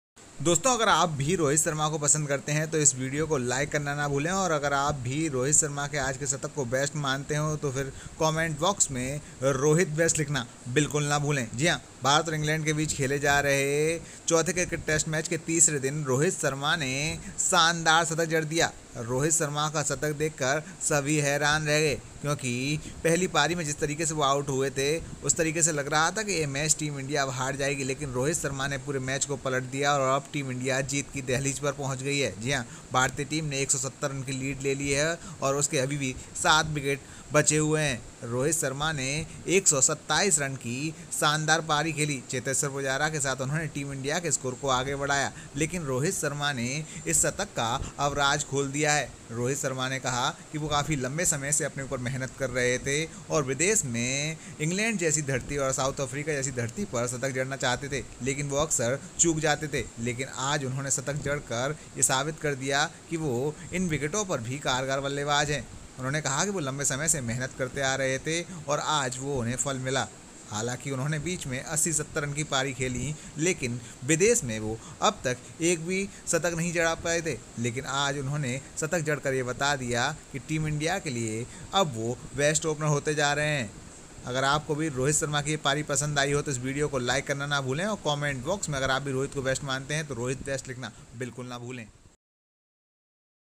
रोहित शर्मा ने खोला शतक का राज, बहुत दिनों की यह मुराद हुई पूरी, देखिये पूरा वीडियो
Rohit Sharma on His Hundred